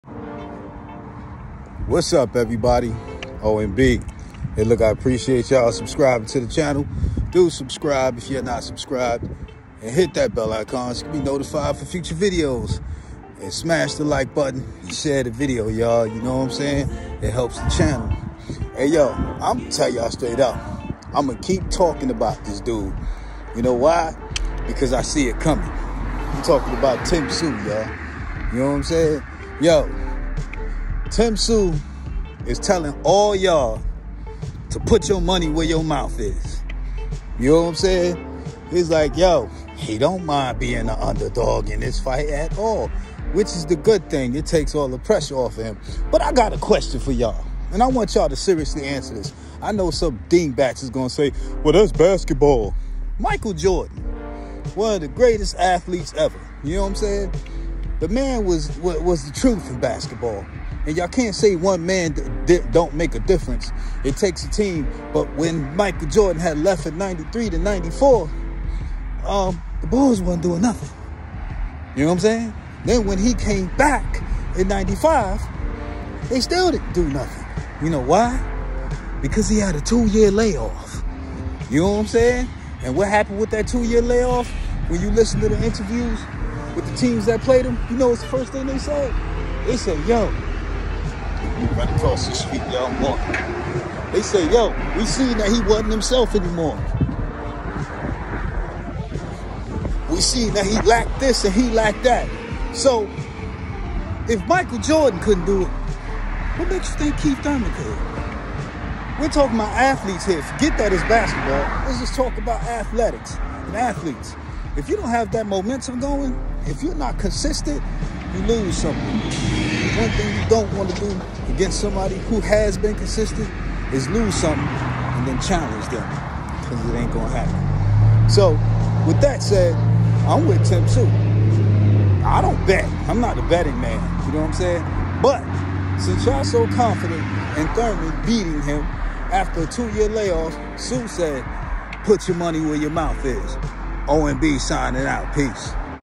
Tim Tszyu Says Put Your Money Where Your Mouth Is 🤫
I’m not a betting man but Tim Tszyu is daring you doubters to bet against him 🫣